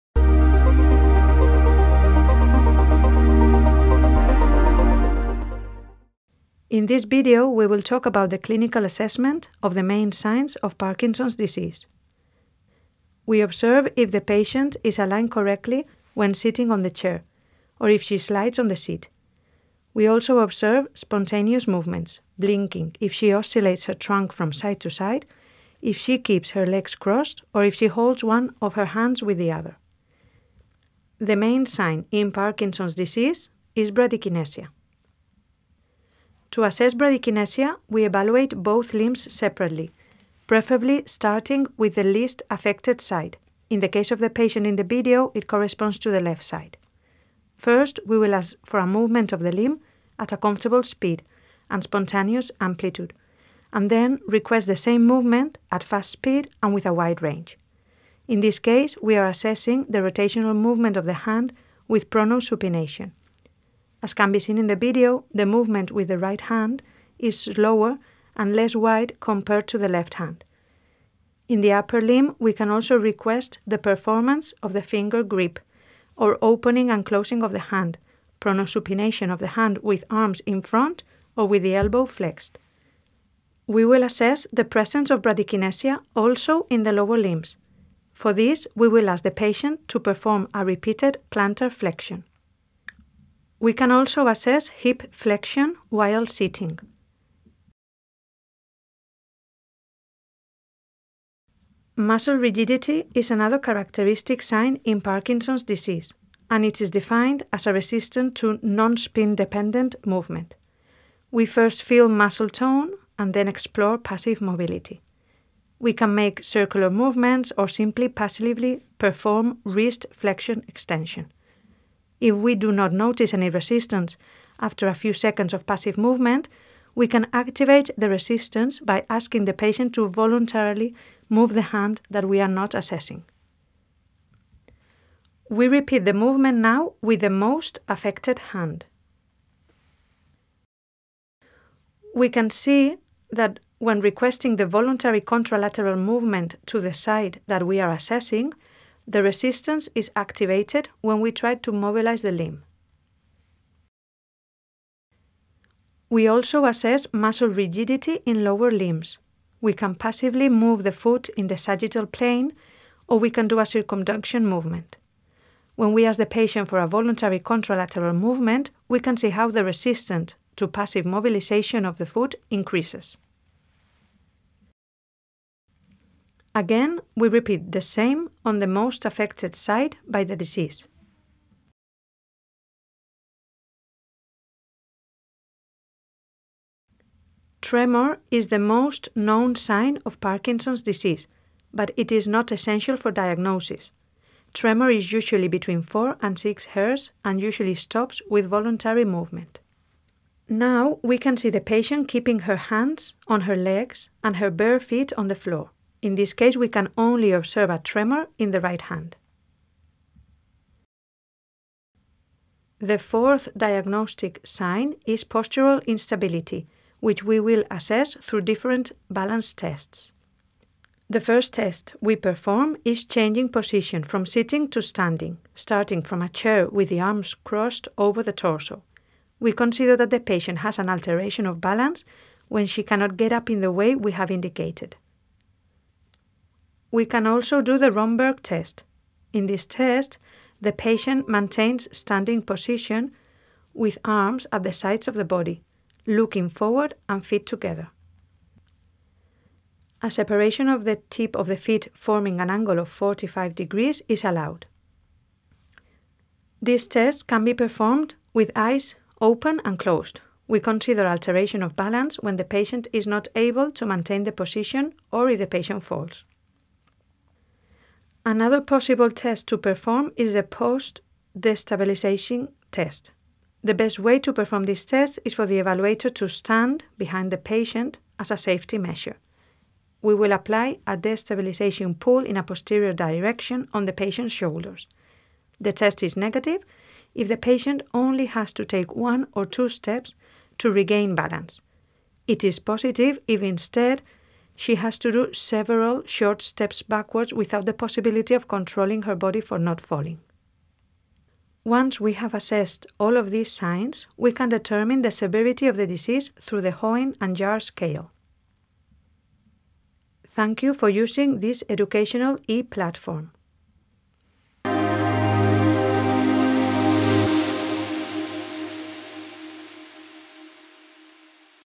Cardinal signs assessment of Parkinson's disease
Authors: Constanza San Martín Valenzuela; Cristina Flor Rufino; María Luz Sánchez Sánchez; Natalia Cezón Serrano; Anna Arnal Gómez A; José M. Tomás.
Description: The content of this video corresponds to the assessment of the clinical signs of Parkinson's disease that allow identifying the severity of the pathology in a person with the disease. Digital resource generated for the platform